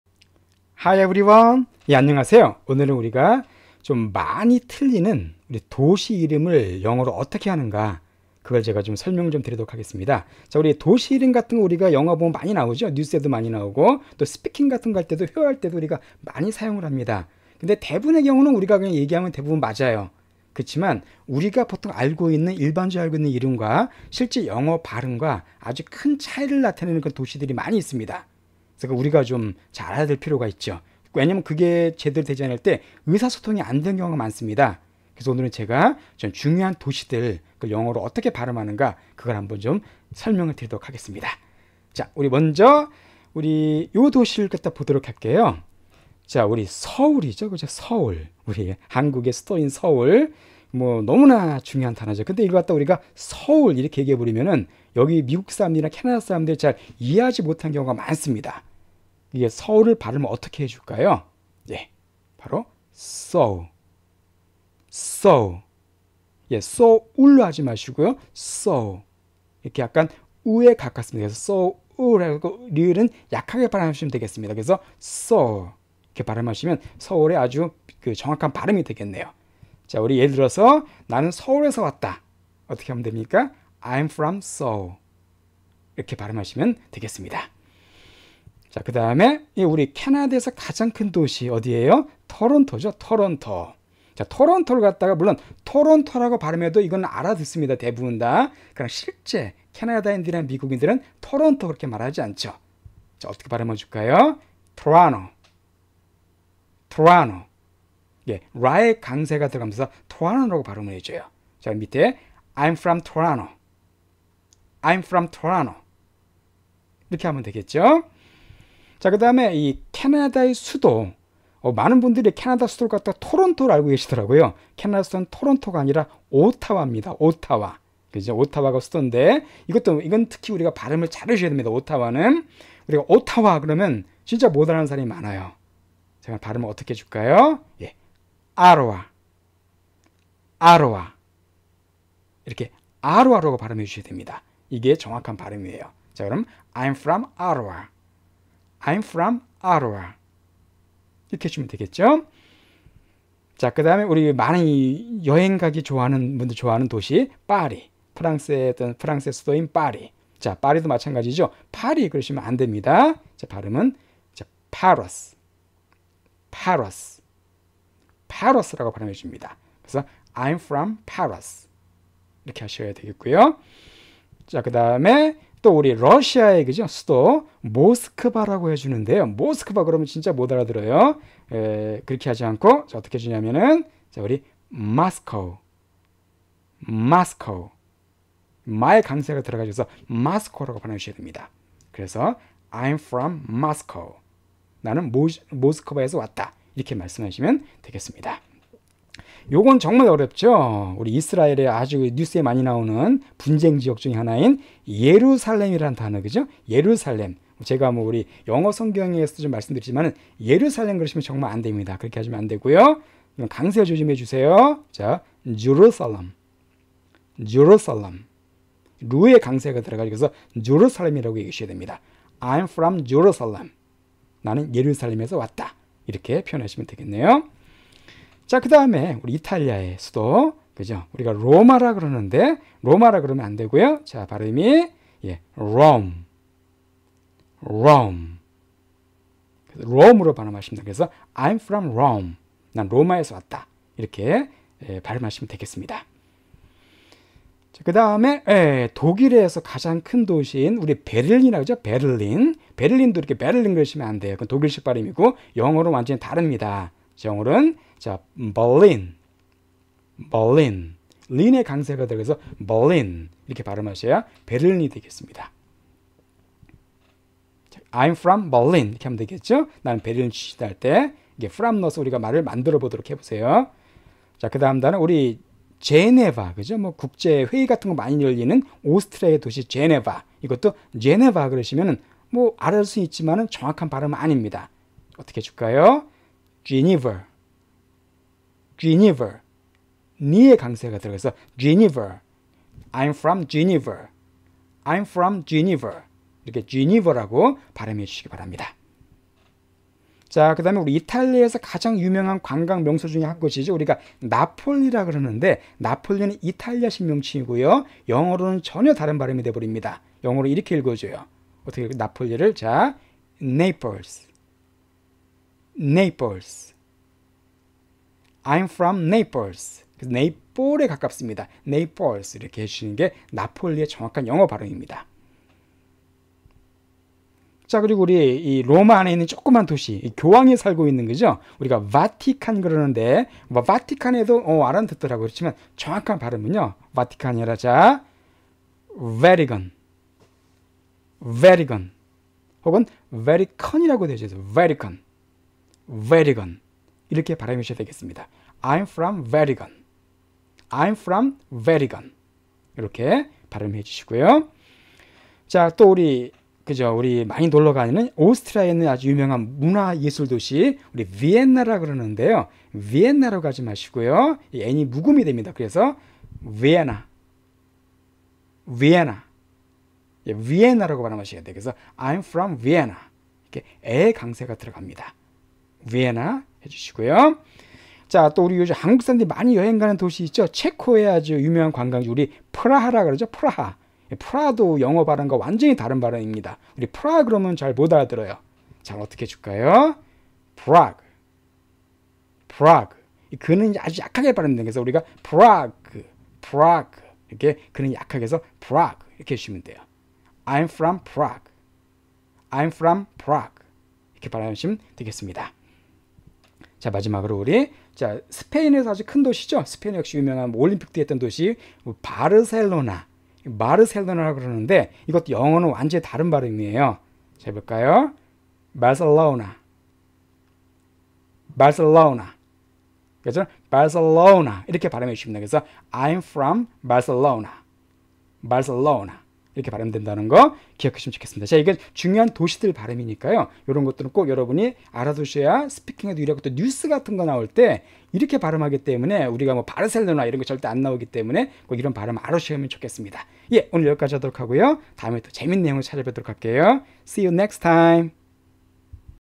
"서울?", "토론토?" 많이 틀리는 도시이름 영어발음
서울, 토론토, 오타와, 프라하, 파리 등 세계 주요도시들의 영어발음을 "한글 표기"대로 읽으면 외국인들은 못 알아듣는 경우가 많습니다. 우리가 특히 많이 틀리는 여러 도시들의 영어발음을 알려드려요.